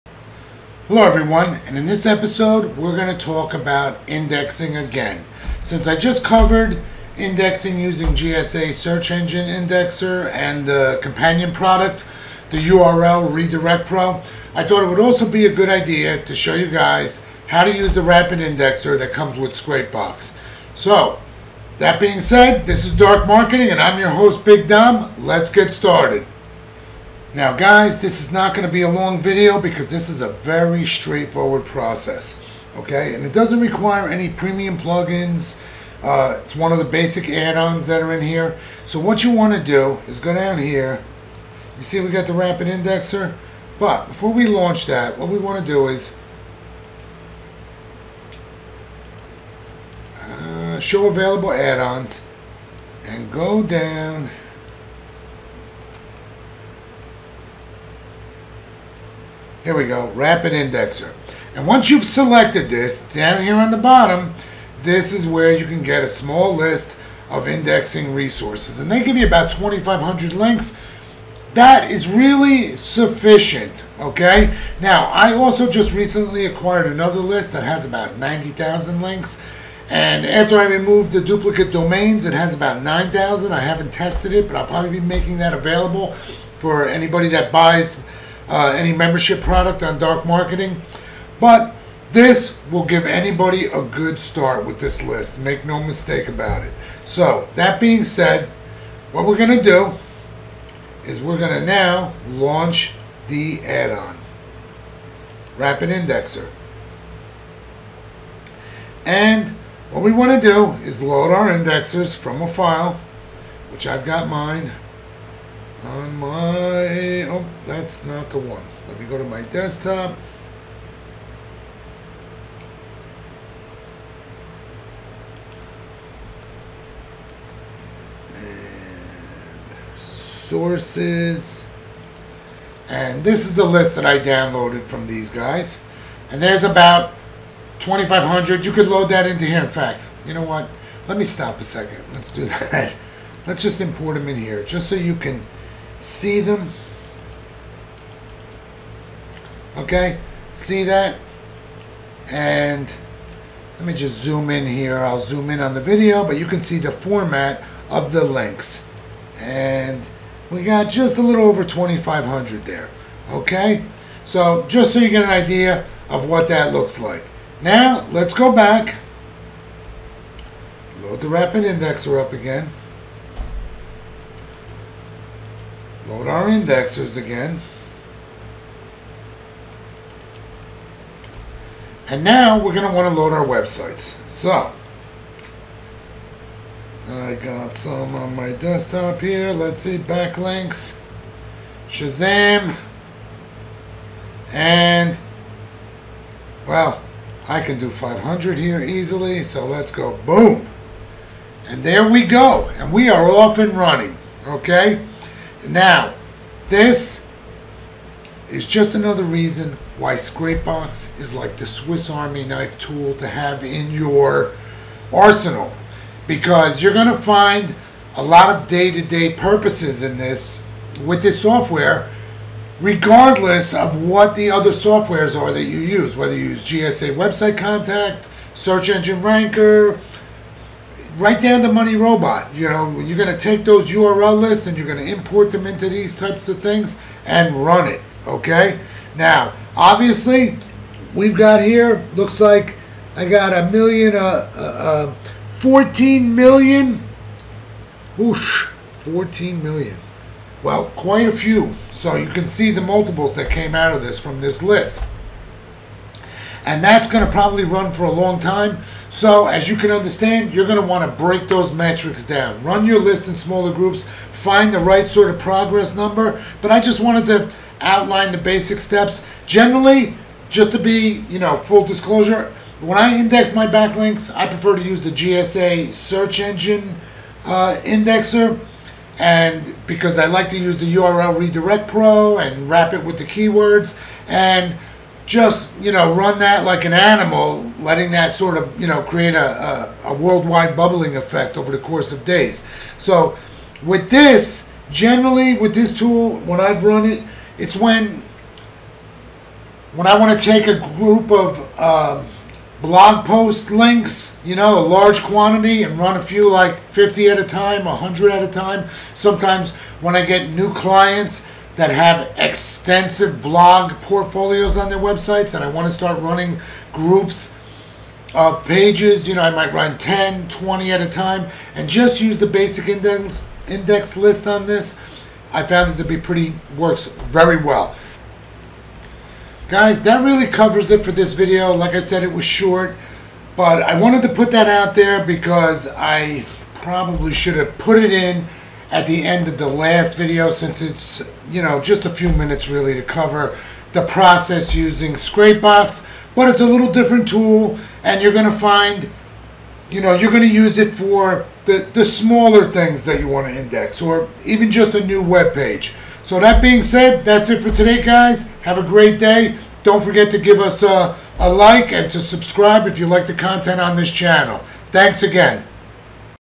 ScrapeBox Rapid Indexer Review and Tutoial Quick Overview
This is just a short video to cover page indexing process, utilizing Scrapebox and it’s rapid indexer add on. I really should’ve included this in the last video about indexing, but it’s probably for the best as it’s easy to look up later, and I’ll be able to put it in a Scrapebox playlist.

⚡Now Available! GSA Website Contact Form Lists to Purchase!